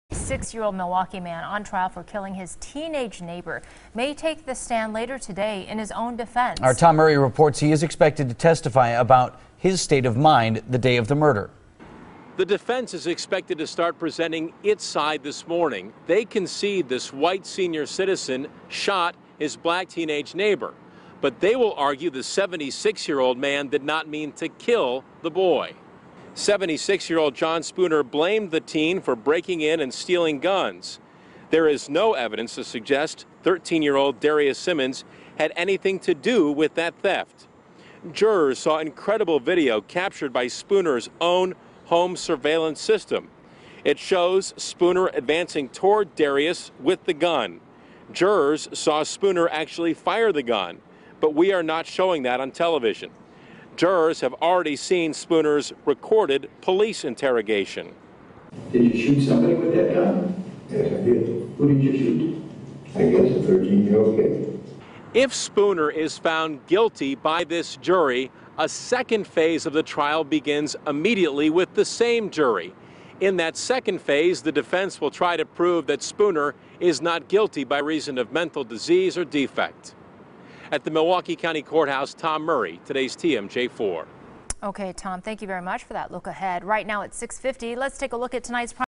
Spooner trial continues Wednesday
The trial continues Wednesday in the case of the 76-year-old man who shot his 13-year-old neighbor.